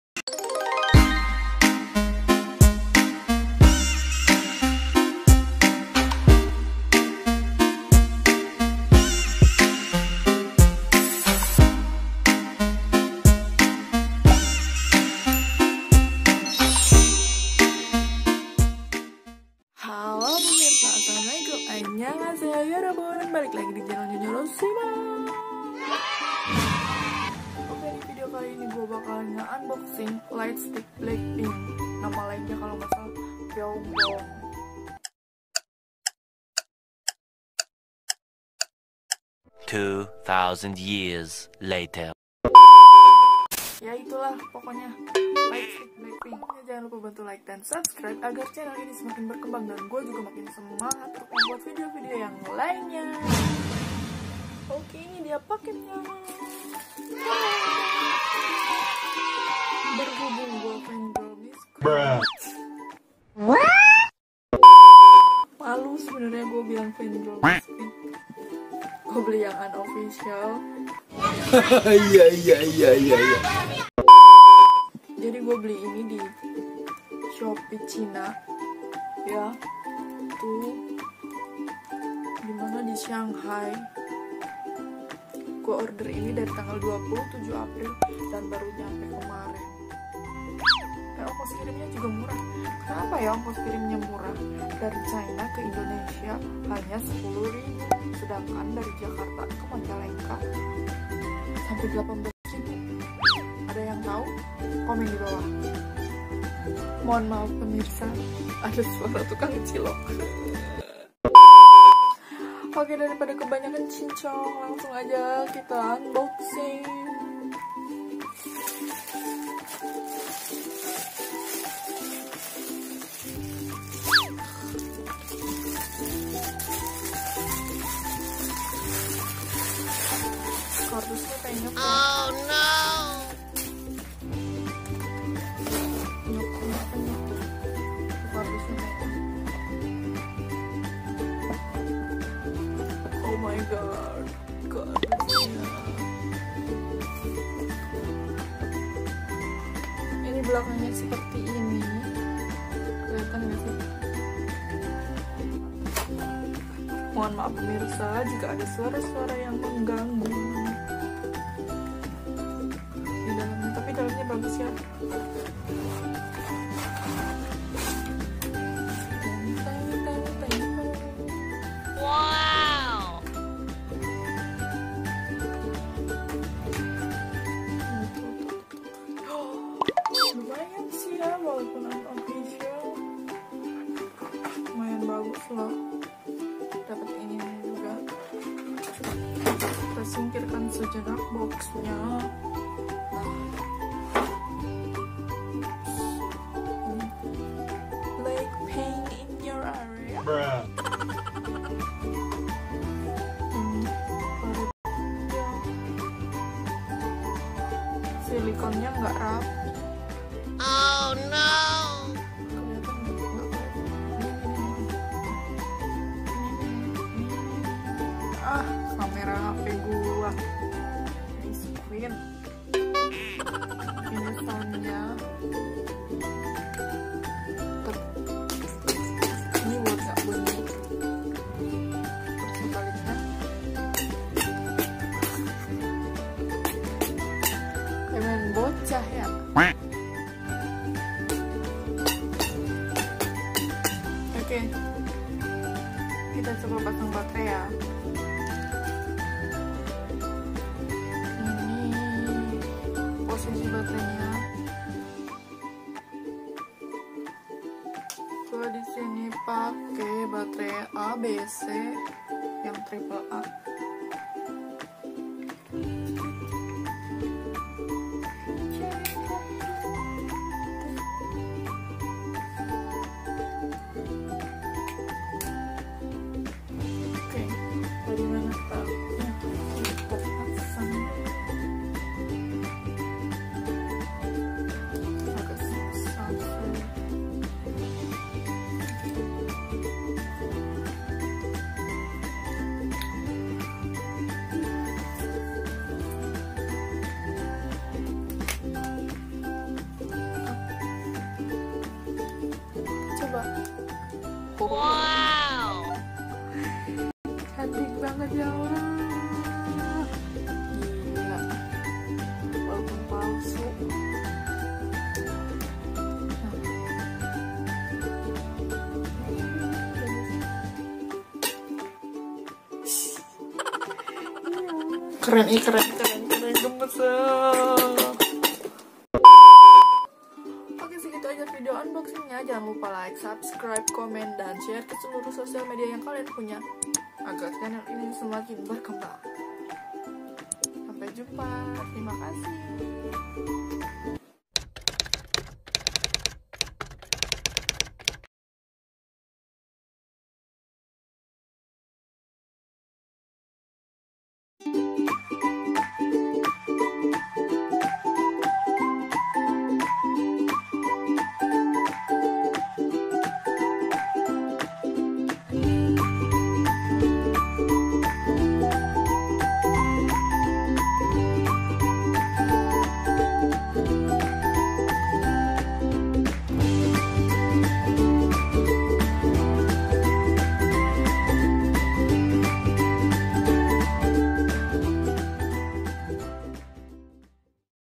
Unboxing Lightstick BLACKPINK (pyong bong) , sambil nunggu buka puasa || Ny.Rasiman
Editing   : √ Kinemaster
                  √ Filmorago

Bantu dukung chanel gue ya temen-temen semuanya, cukup dengan

🙏🙏🙏...Terimakasih...🙏🙏🙏